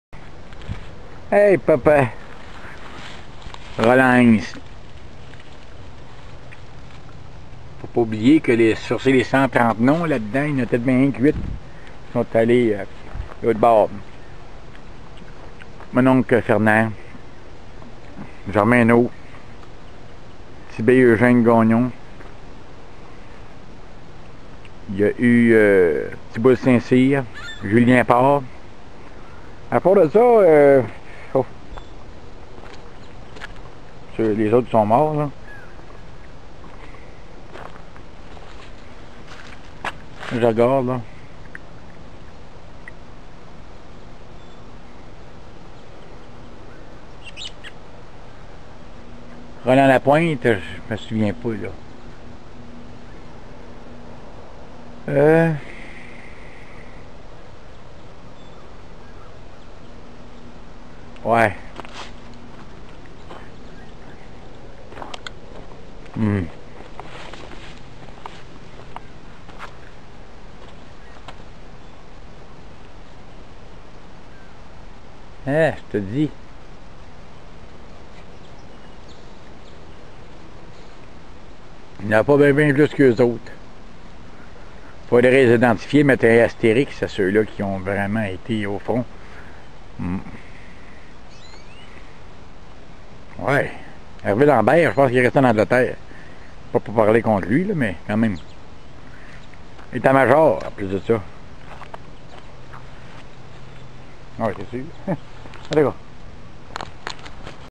Les heros de Brompton
Les valeureux anciens combattants de chez nous qui on été au front